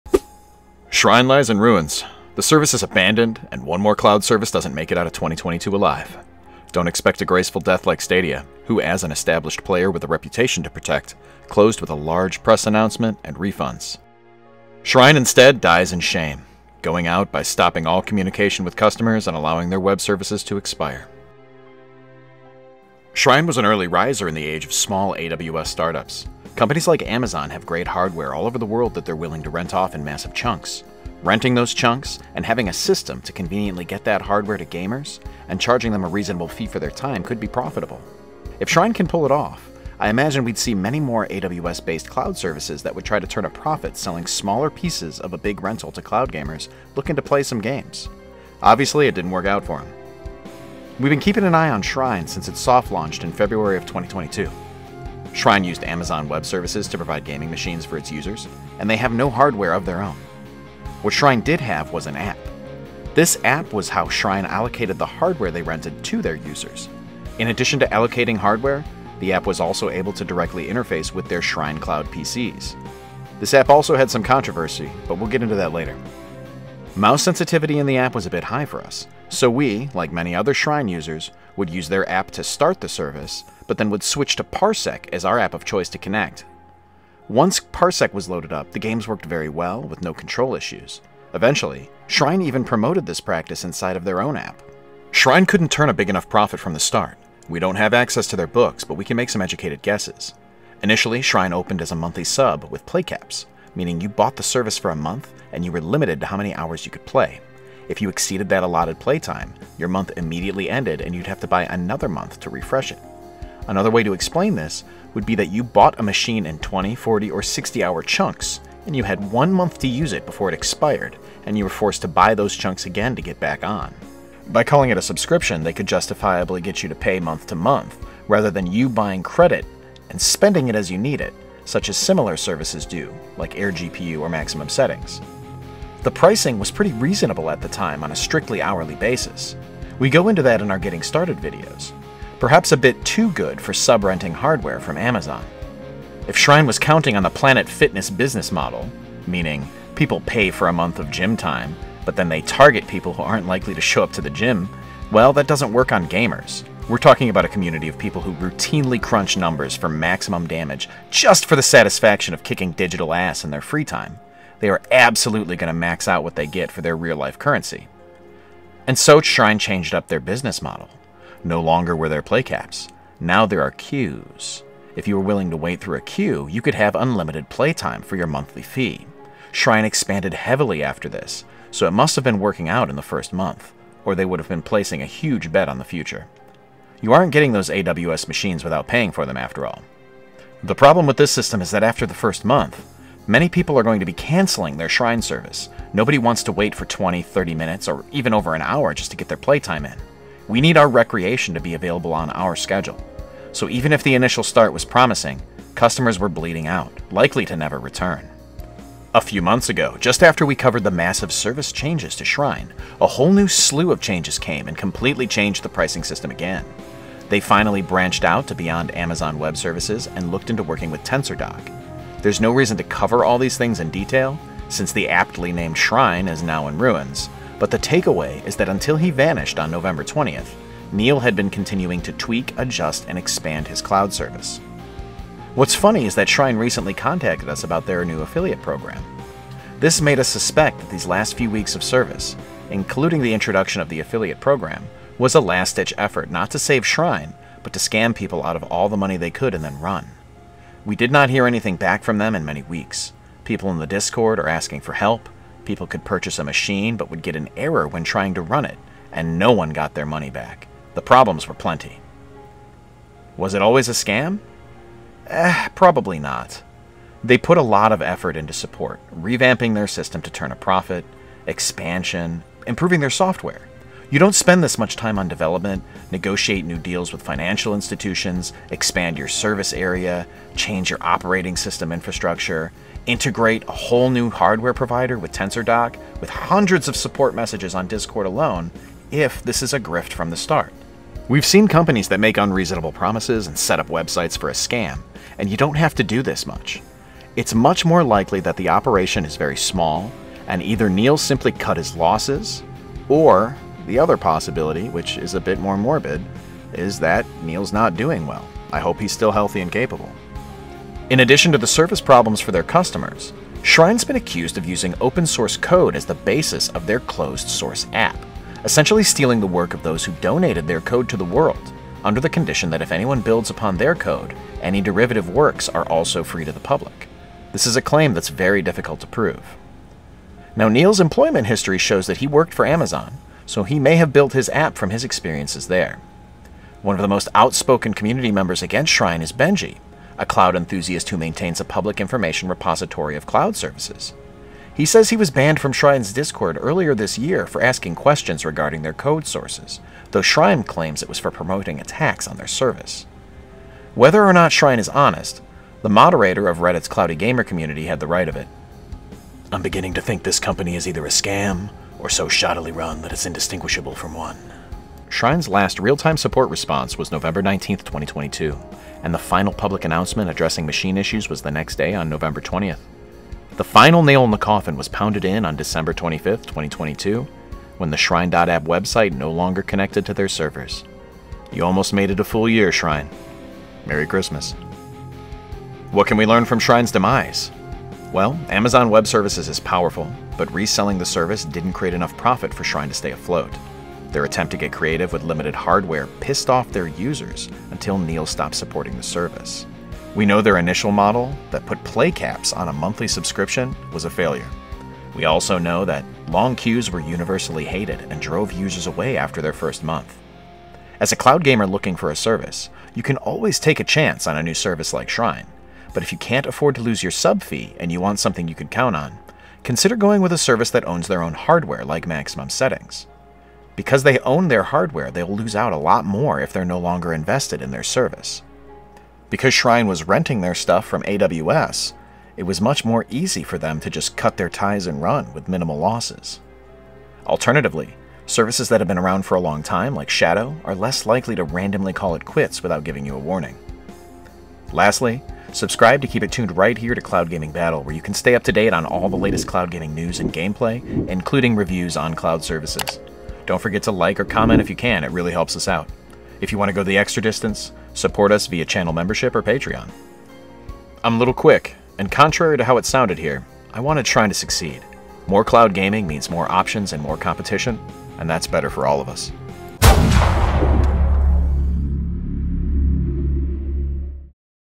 SHRINE Cloud Gaming is DEAD!
Shrine.app lies in ruins. Another cloud gaming platform is gone. This time without notice. Shrine up and abandoned the entire platform weeks ago without even letting their customers know that they are shutting down. Details here.